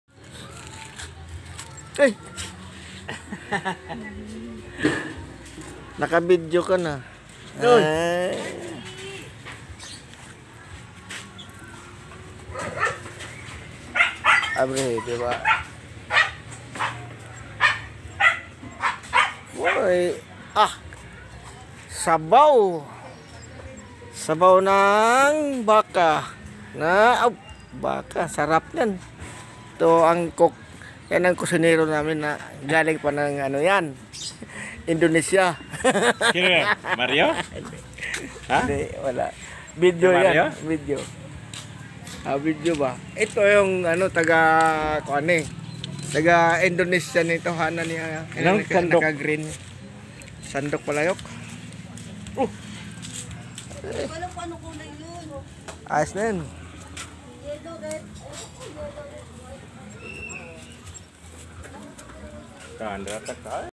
Karning Baka Nilaga || Bagong gawa kong kalan # Joemaps Tv
# ginawaan ko si George
# concrete kalan ginawa ko para
# maglaga ng baka.